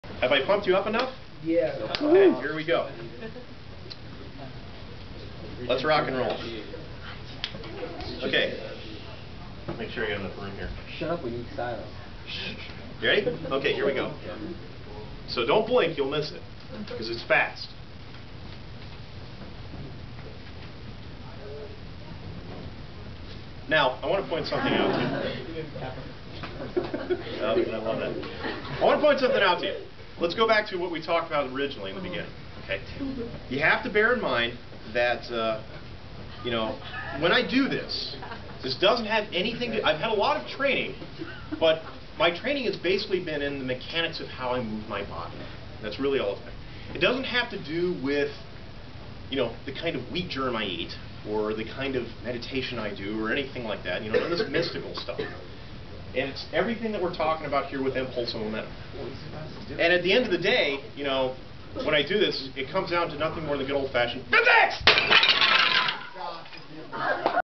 Physics of Board Breaking
This is footage from a physics demonstration I did for my students. We had been studying the physics of collisions and how impulse and momentum play a role in karate board-breaking tricks. It all really just boils down to impulse = change in momentum (or F*t = delta(mv) for you math folk).

It might surprise many people to know that pretty much anyone can break boards with little training - in order to do more challenging breaks like that pictured here takes more training, though the basic principles are still the same. But in the end there is *nothing* mystical involved - no chi or "life energy" or any of that rot. With proper body mechanics and good use of mass (twisting the body), velocity (dropping to convert GPE into KE), and a low time of impact (solid supports that won't give) one can make lots of kindling out of boards.  Oh yeah, and the spacers are a nice trick as well :)

Here's a great article from Discover magazine which goes into more detail about the physics of karate strikes...